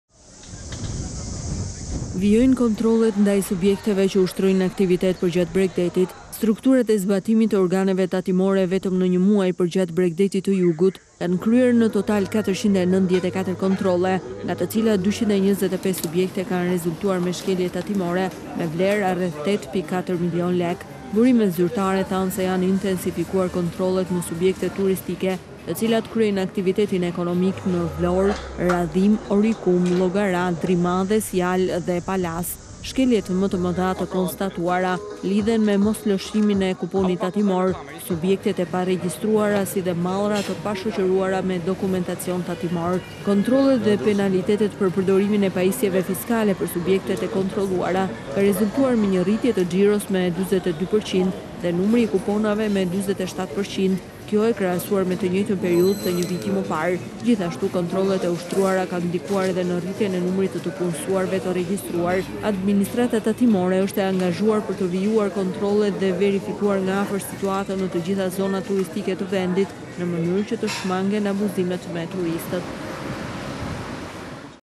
Ora News - Në sitë bizneset përgjatë bregdetit, 494 kontrolle
Vijojnë kontrollet ndaj subjekteve që ushtrojnë aktivitet përgjatë bregdetit. Strukturat e zbatimit të organeve tatimore vetëm në një muaj kanë kryer në total 494 kontrolle, nga të cilat 225 subjekte kanë rezultuar me shkelje tatimore me vlerë rreth 8.4 milionë lekë.

Burimet zyrtare thanë se janë intensifikuar kontrollet në subjektet turistike që kryejnë aktivitet ekonomik në Vlorë, Radhimë, Orikum, Llogara, Drimadhes, Jalë dhe Palasë.shkeljet më të mëdha të konstatuara lidhen me moslëshimin e kuponit tatimor,subjektet e paregjistruara si dhe mallra të pashoqëruara me dokumentacion tatimor.

Kontrollet dhe penalitet për përdorimin e pajisjeve fiskale, për subjektet e kontrolluara ka rezultuar rritje e xhiros me 42% dhe numri i kuponave me 47%, kjo krahasuar me të njëjtën periudhë të një viti më parë. Gjithashtu kontrollet e ushtrura kanë ndikuar edhe në rritjen e numrit të punësuarve të regjistruar.

Administrata Tatimore është e angazhuar për të vijuar kontrolluar dhe verifikuar nga afër situatën në të gjitha zonat turistike të vendit në mnyrë që të shmangen abuzimet me turistët.